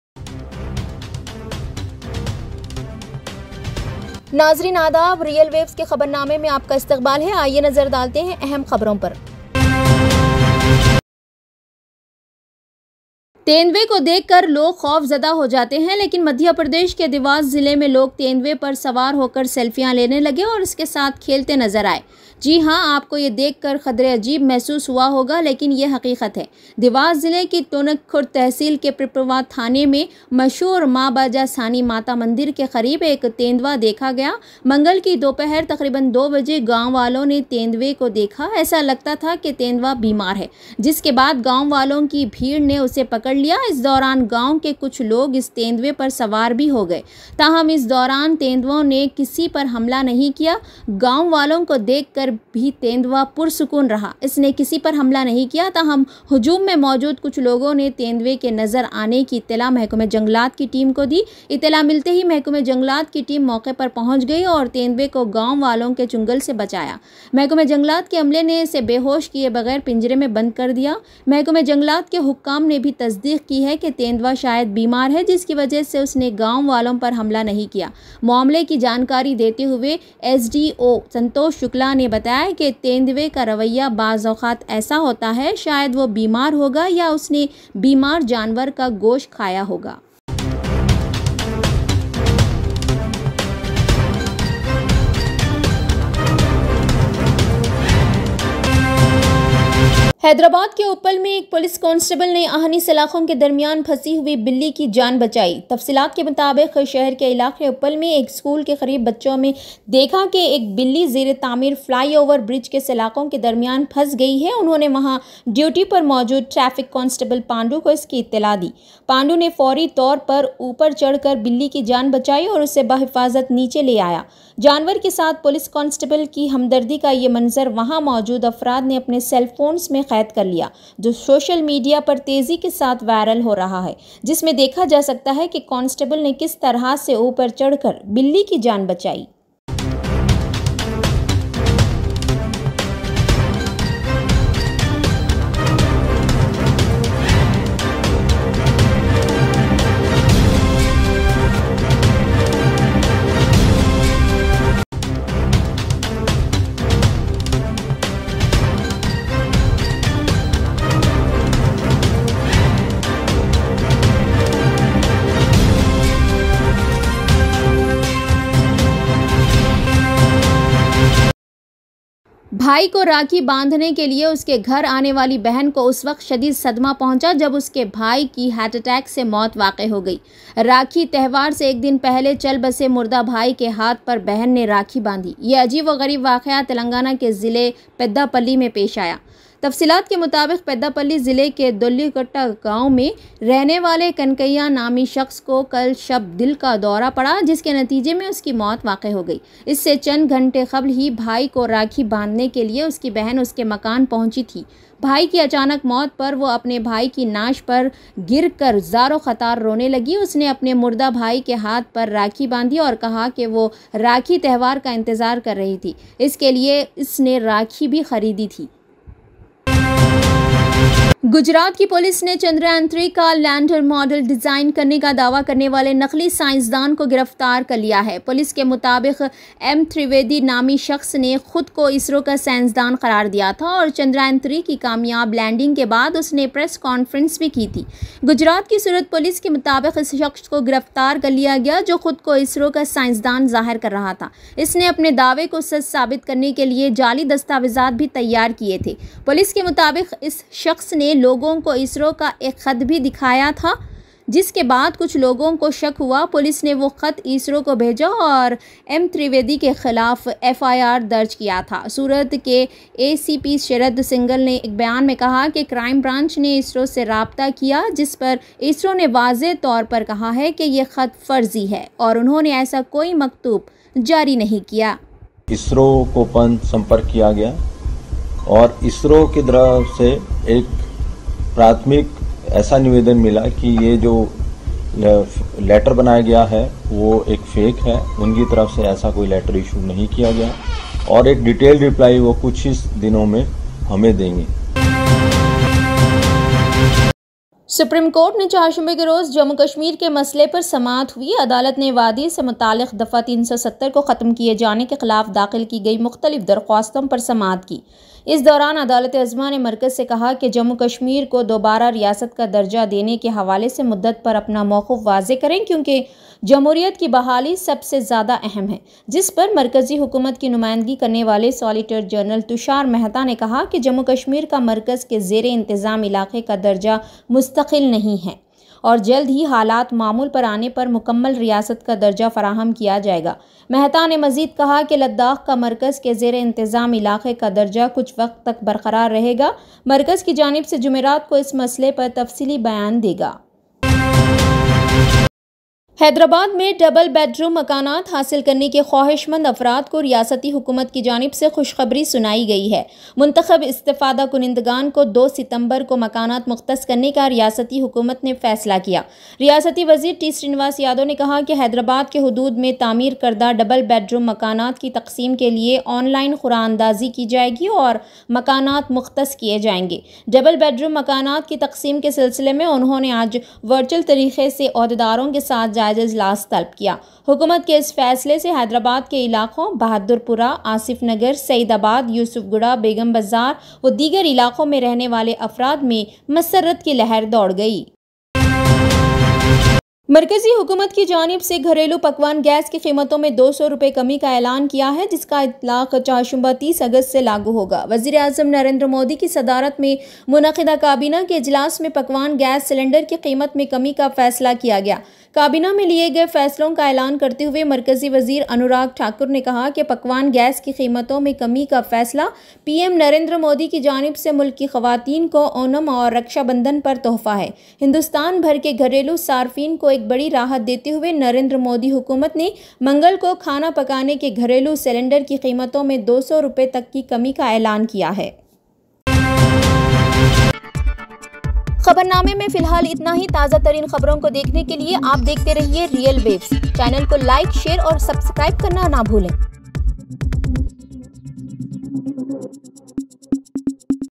TODAYS FULL NEWS ! 30-08-23 ! WATCH TOP HEADLINES ! ONLY ON REALWAVES !
isro ki science dhano nai chand par oxygen ki maujudgi aur muktalif malmuniyad chandyaan3 se kaha!
hyderabad mai double bedroom makanat hasil karne kai afraad ko hukumat nai kush kabhri sunayi !
markazi hukumat ki janib se ghairelu pakawan gascylinder ki kematou mai 200rupeekami kiya gaya hai!
supreemcourt nai wednesday ko jammukashmir kai masle par samapt hui !
Madhya Pradesh Leopard Video Viral!
Fake ISRO Scientist Arrested: Surat Man Poses as ISRO Scientist, Claims Chandrayaan 3 Design!